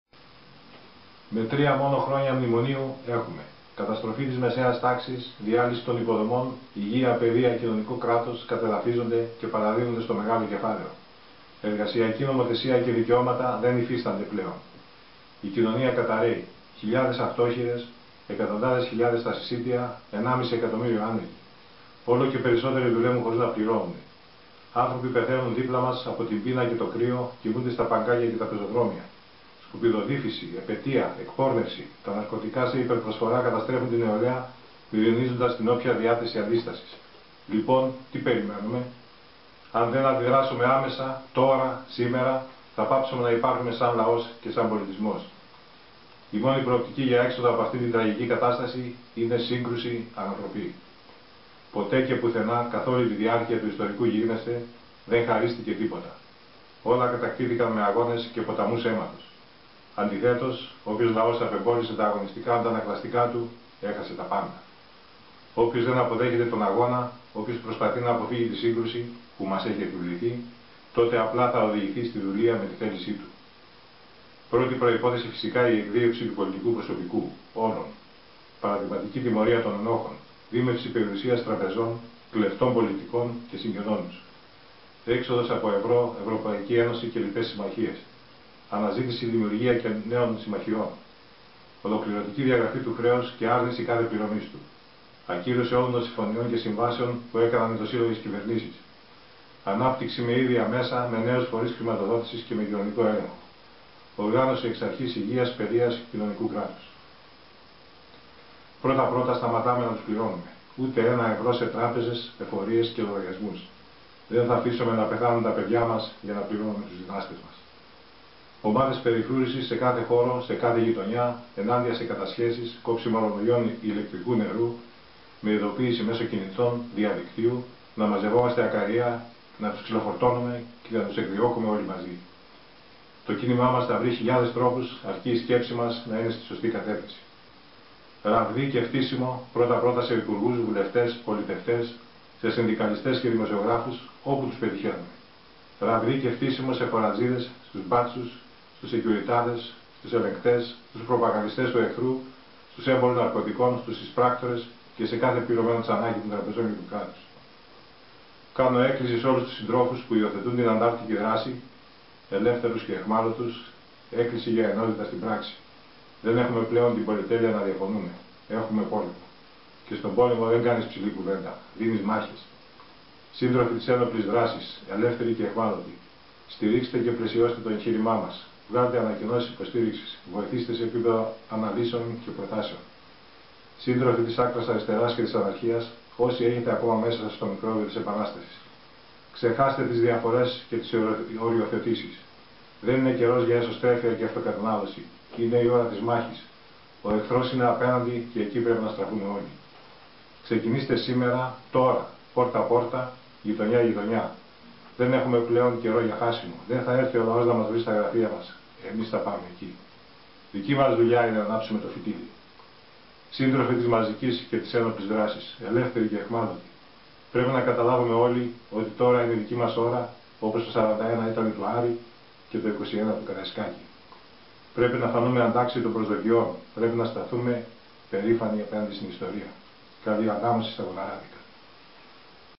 Χριστόδουλος Ξηρός Βίντεο
Με απειλές κατά πάντων και υπογραφή «Χριστόδουλος Ξηρός, ελεύθερο μέλος της 17Ν» ο καταζητούμενος δραπέτης έκανε διαδικτυακά την εμφάνισή του. Συνελήφθη στην Ανάβυσσο με βαμμένα ξανθά μαλλιά του και μούσι